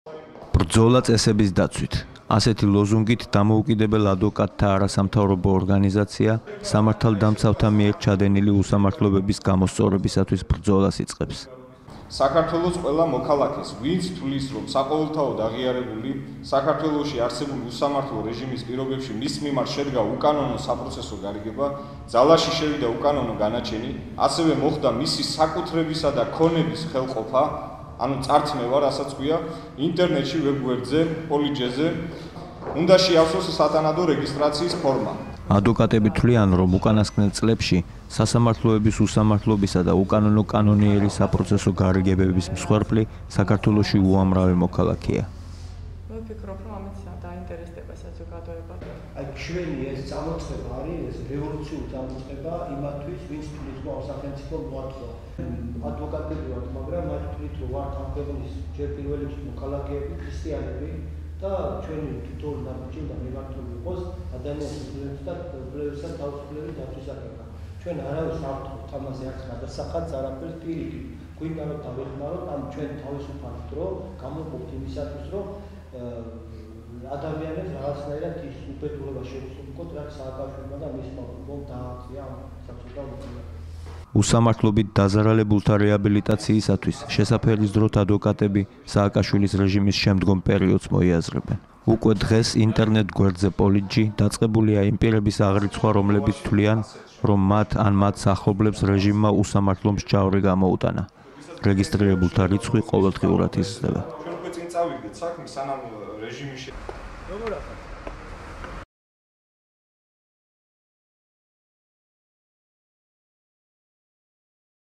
კანონიერი ბრძოლა უკანონობასთან
ადვოკატები თვლიან, რომ ბოლო წლებში, საქართველოში, უსამართლო განაჩენისა და თავსმოხვეული საპროცესო გარიგებების მსხვერპლი უამრავი ადამიანია